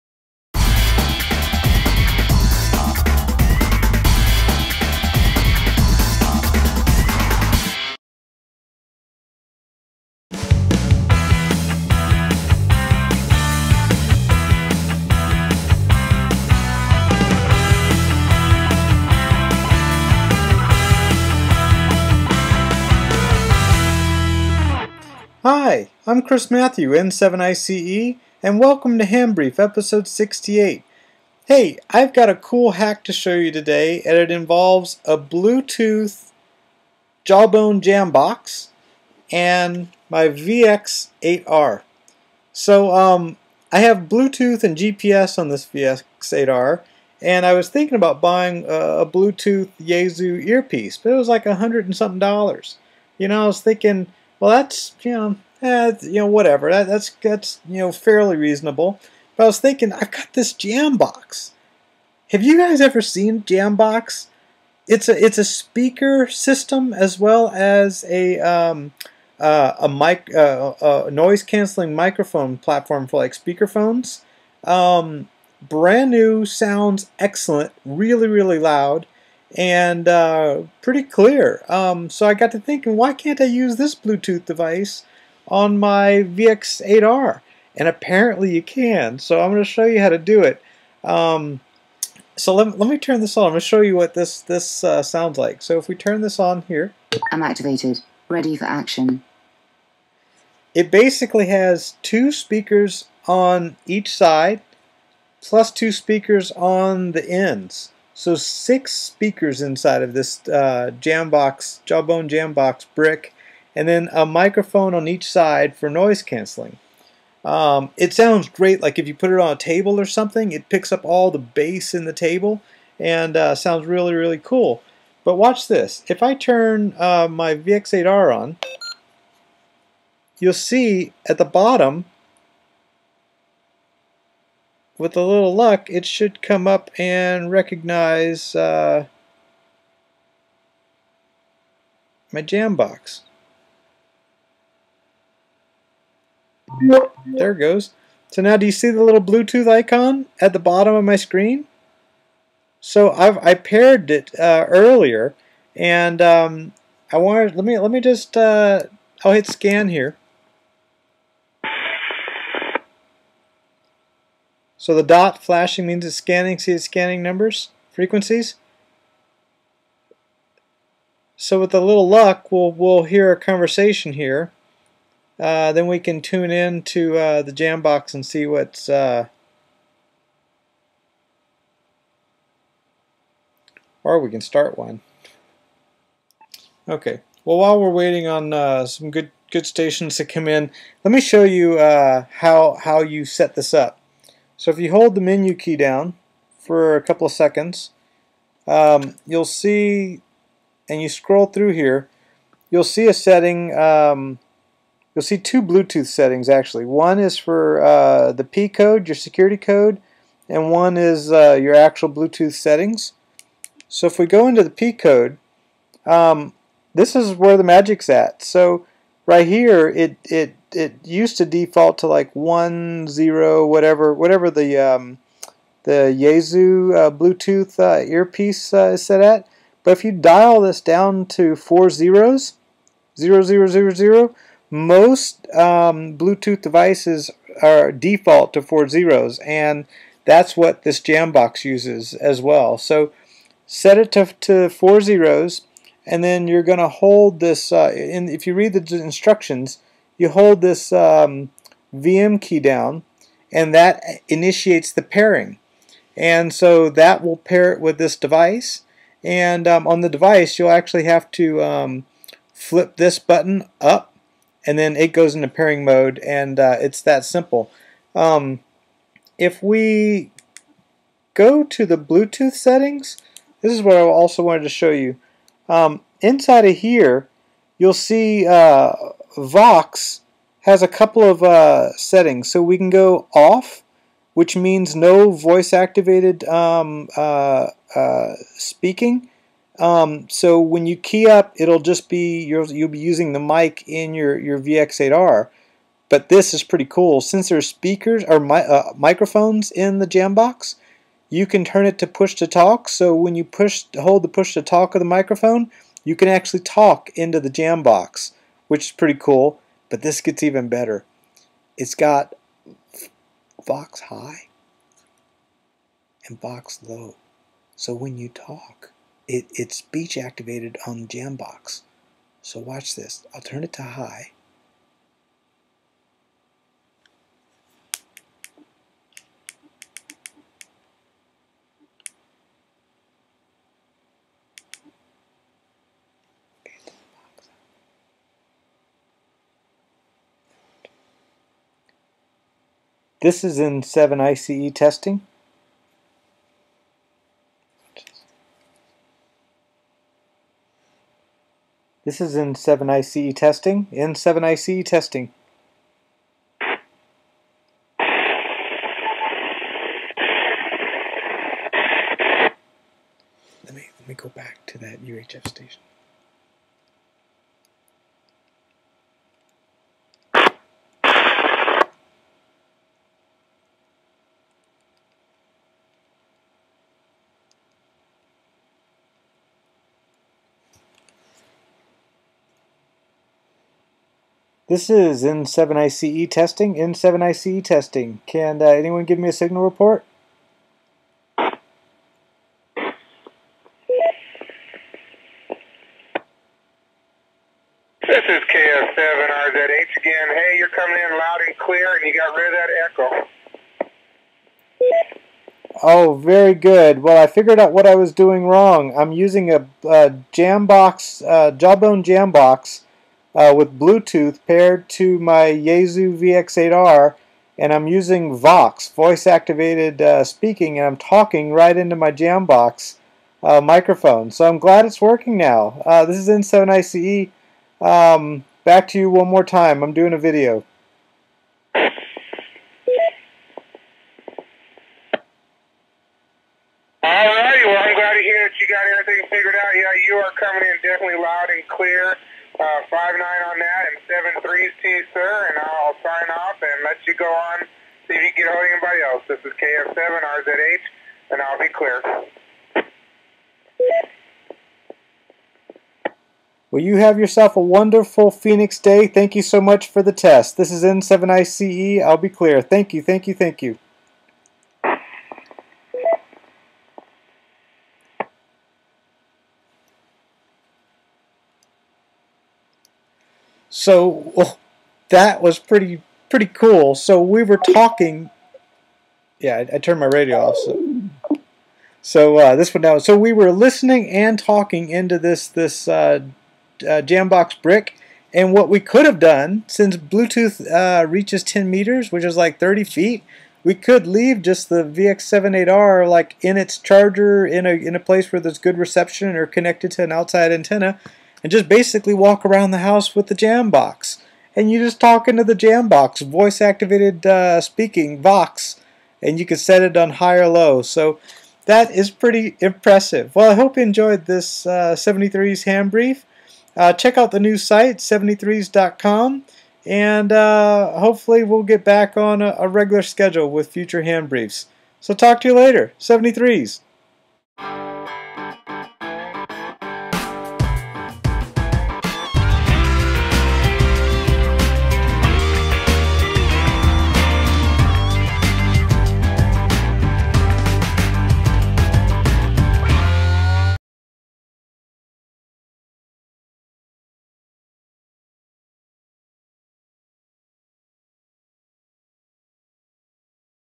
HamBrief 68
Chris Matthieu, N7ICE, demonstrates how to connect the JawBone Jambox to a Yaesu VX-8R using Bluetooth with voice-activated VOX!